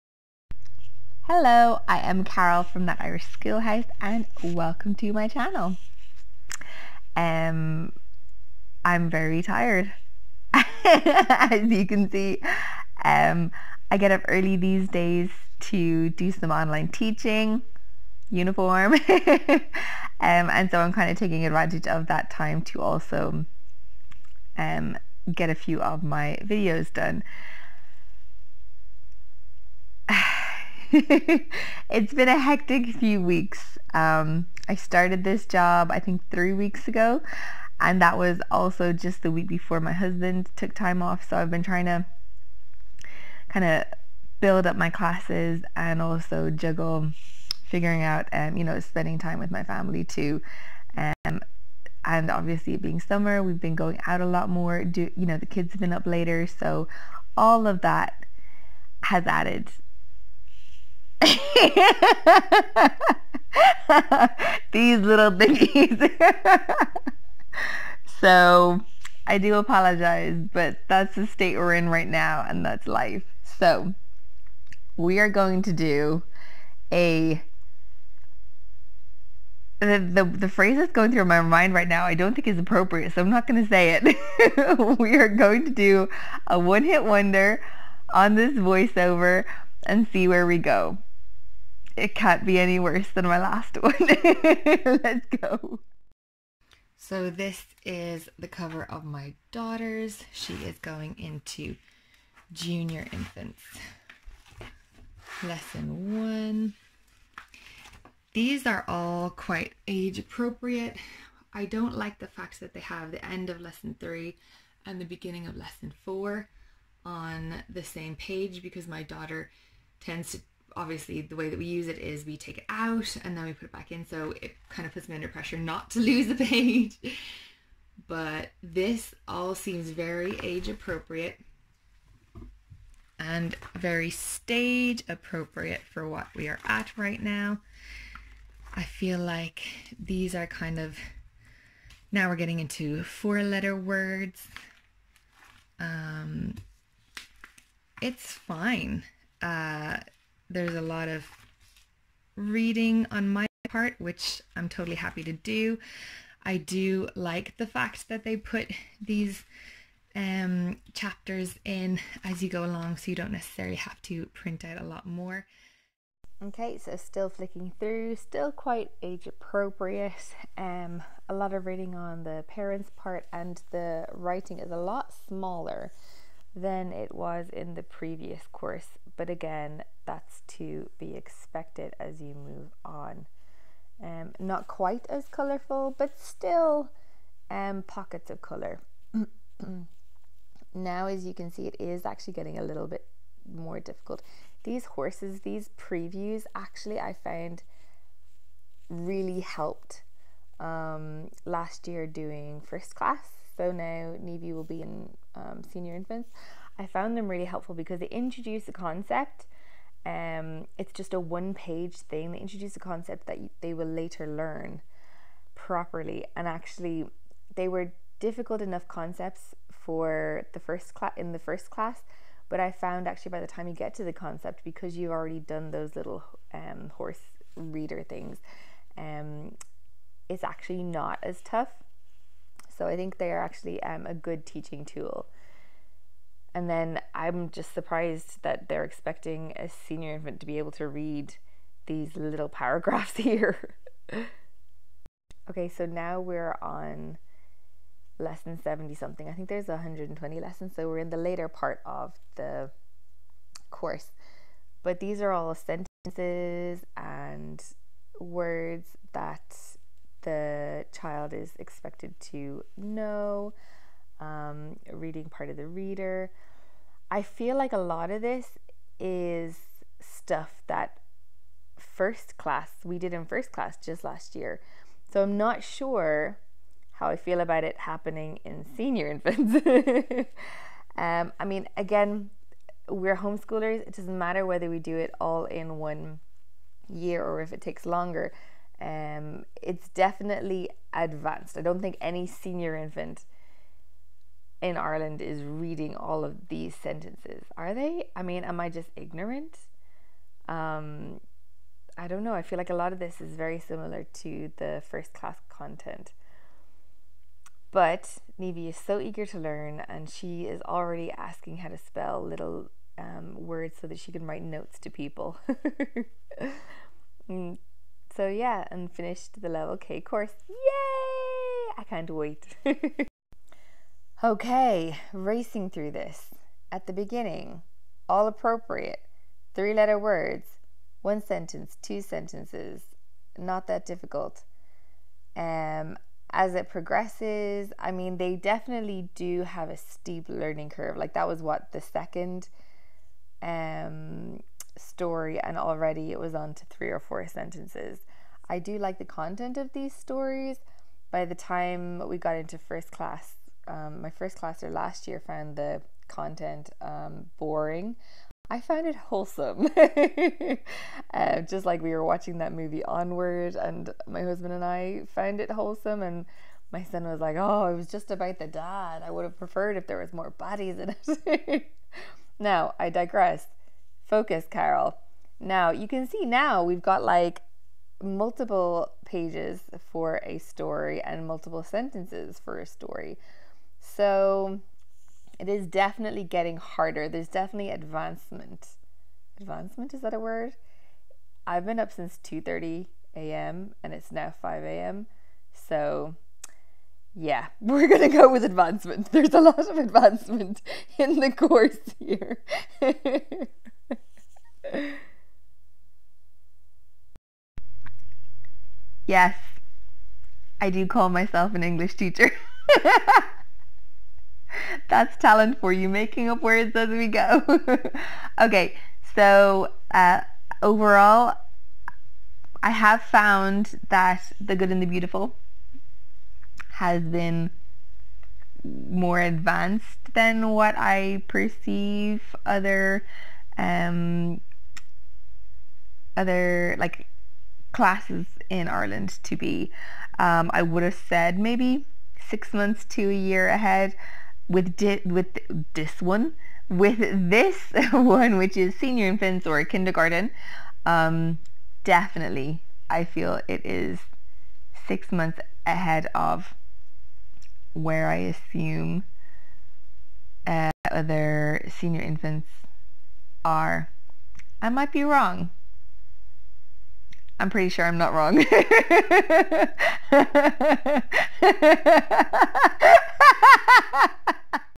TGTB kindergarten language arts (senior infants) curriculum flick through
Flicking through LA K and my initial thoughts.

Excuse the cringe jokes, sometimes I fancy myself as a comedian but I really think it's only me that is entertained by it all 😂

Also I looked it up and advancement IS a word